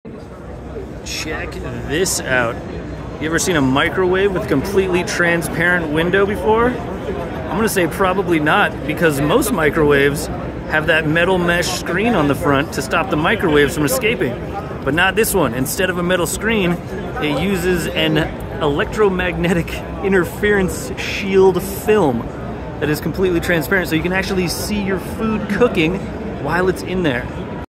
A Fully Transparent Microwave?
We spotted this microwave at CES that has a fully transparent window. Thanks to a new kind of electromagnetic interference shielding film, it doesn't need a metal grate like most microwaves do.